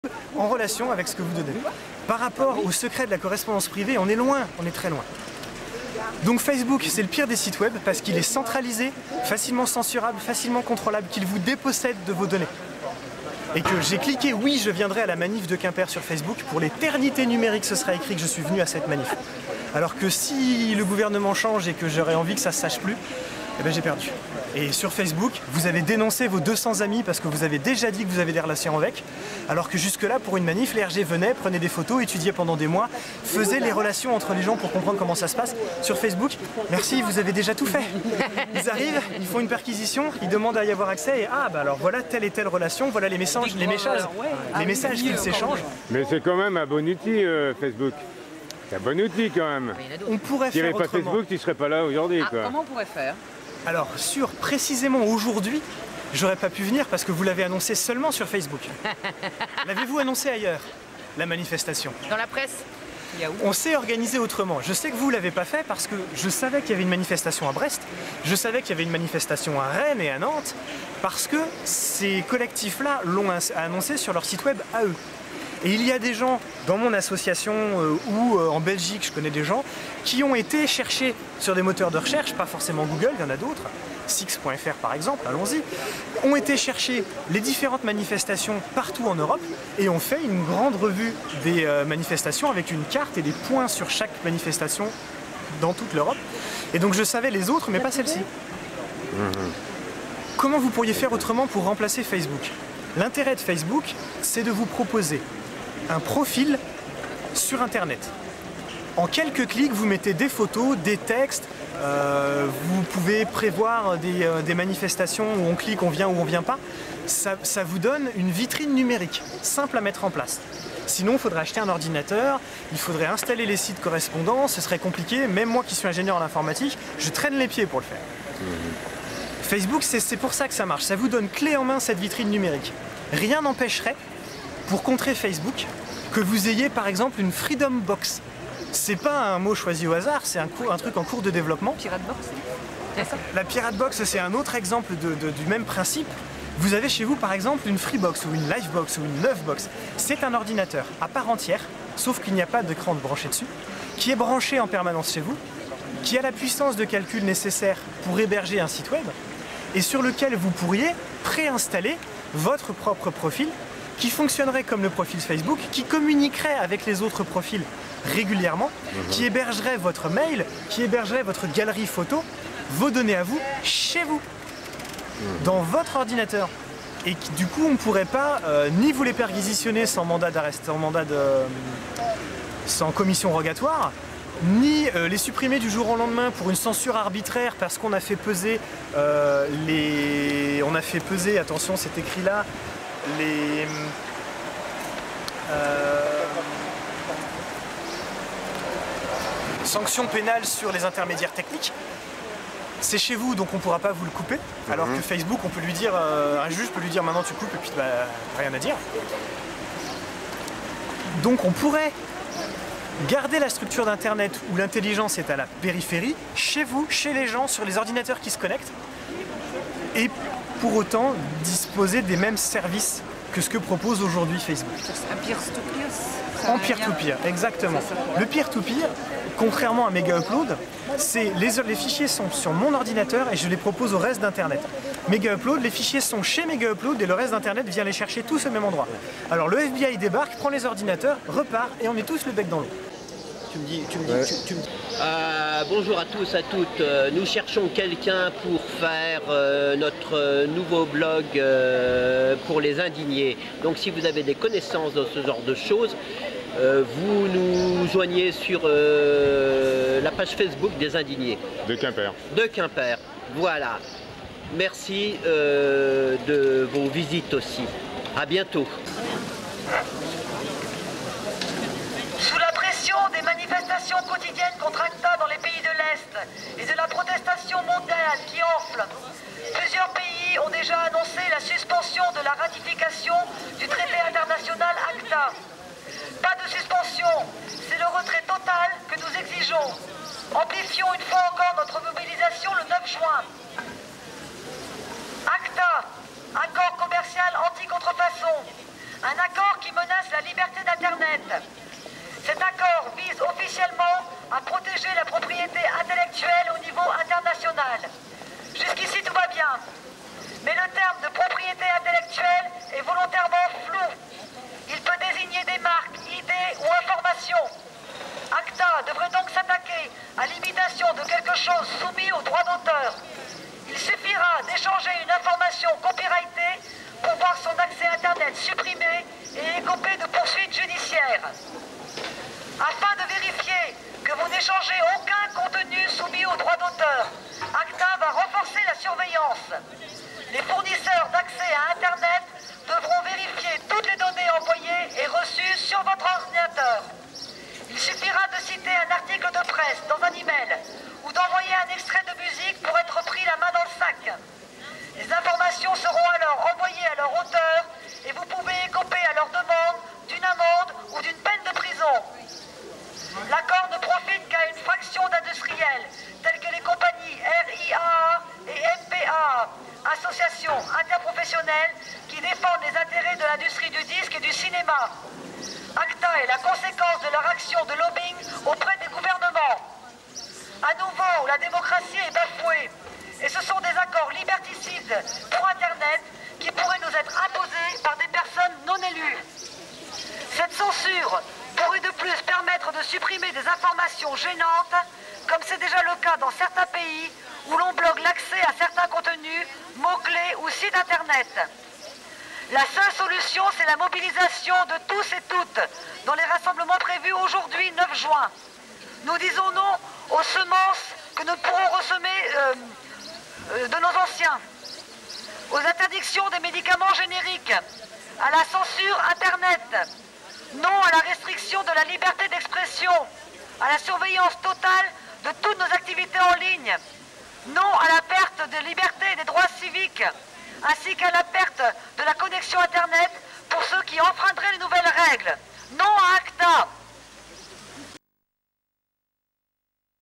stop ACTA Quimper 9 juin 2012 (2ème partie)VTS_01_2
Les indignés d'Quimper/Cornouaille contre le traité ACTA le 9 juin 2012...